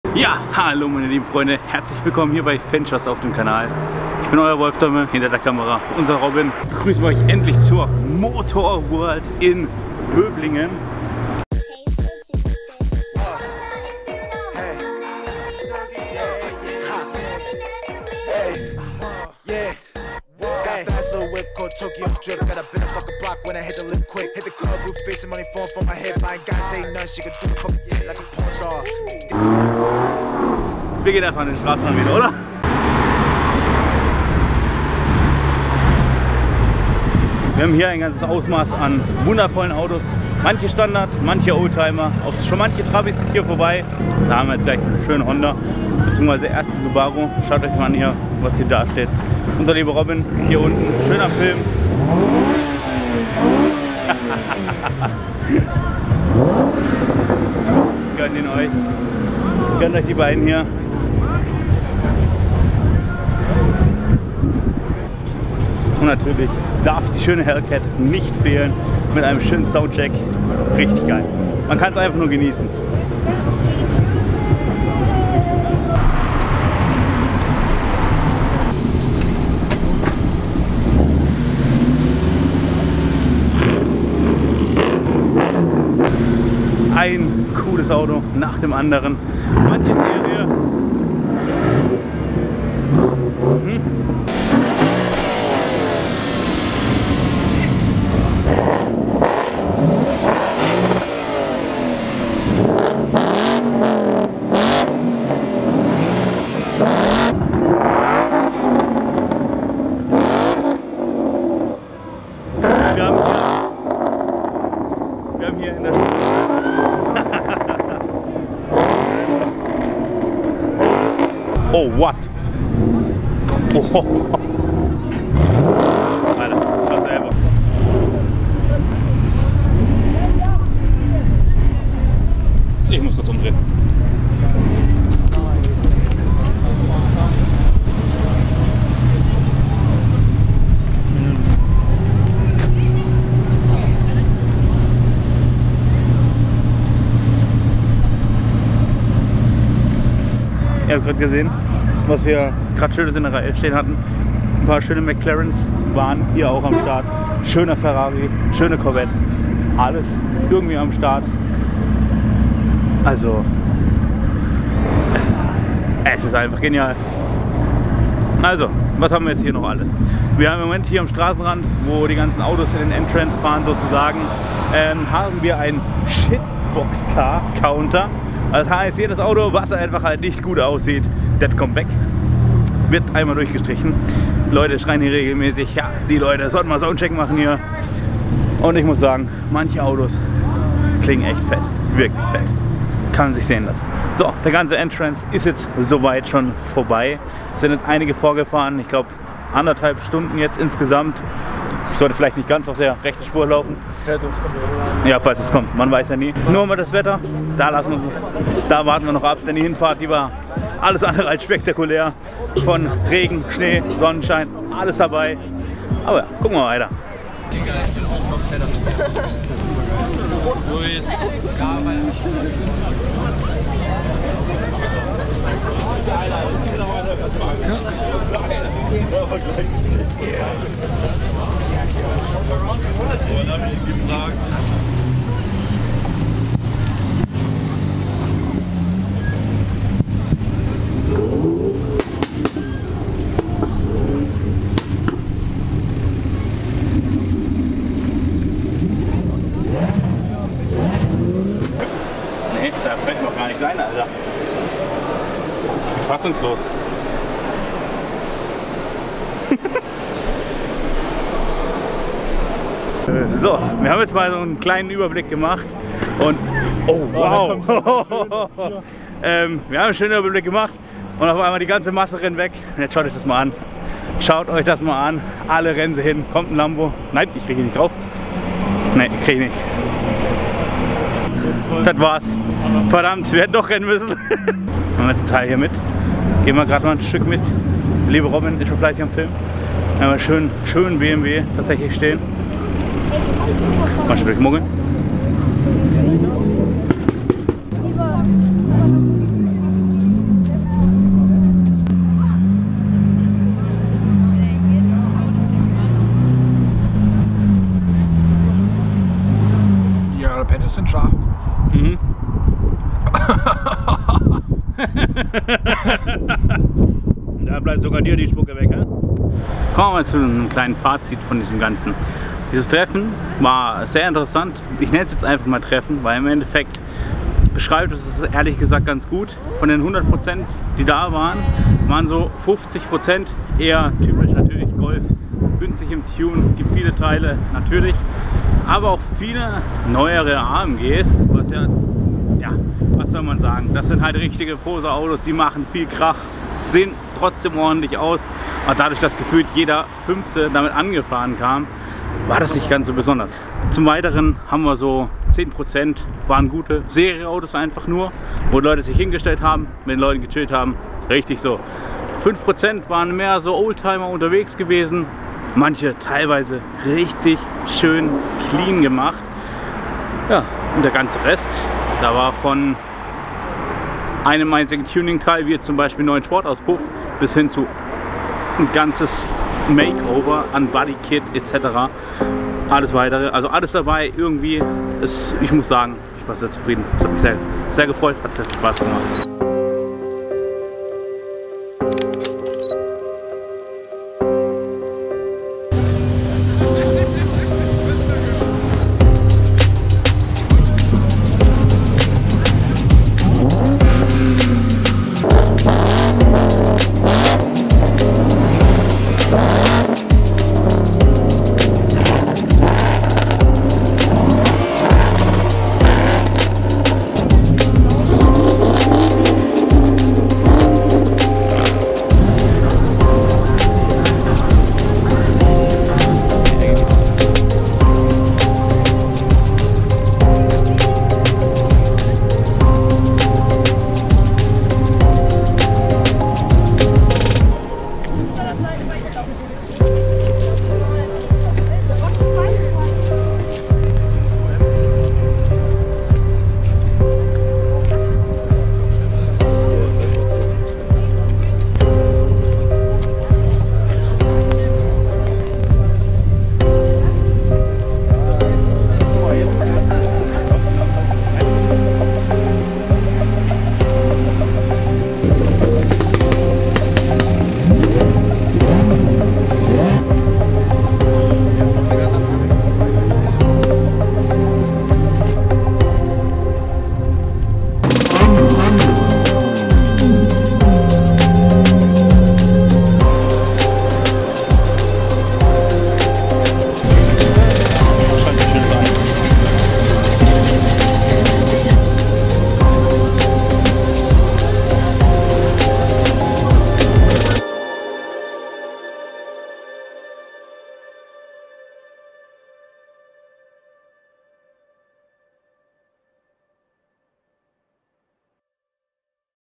MOTORWORLD Böblingen - Der VLOG [2024] Saisonstart & Supercars⛽️
Ein unglaublicher erster Saisonauftakt der Motorworld Böblingen, nähe Stuttgart.

► Ein LIKE würde helfen das Video besser im Youtube-Algorithmus einzuranken!
► Über deinen Support würde ich mich freuen!